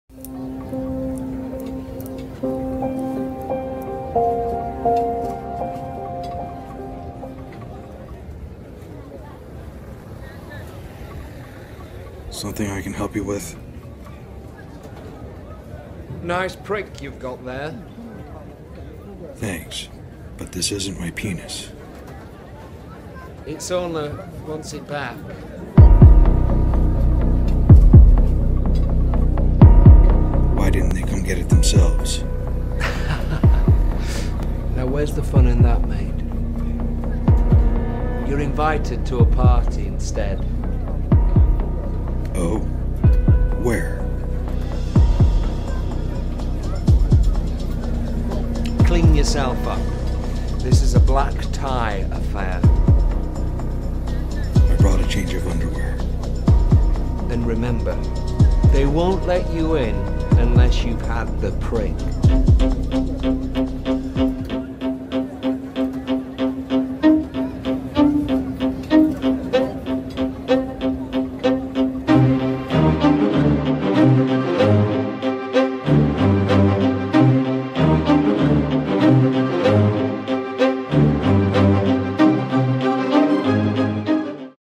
Invited to a Party - ManHeat: The Fool's Gauntlet - Ep 9c - Web Series
Invited to a Party - ManHeat: The Fool's Gauntlet - Episode 9c - Funny Zero-Budget Action/Adventure Web Series

After his brush with death, our hero receives a mysterious invitation from a sketchy English gentleman who wants to invite him to a party. Enjoy!
This episode features a terrific performance by guest star Andrew Shire of @wombledtv. Be sure to drop them a follow!
Music and stock footage courtesy of media library subscriptions.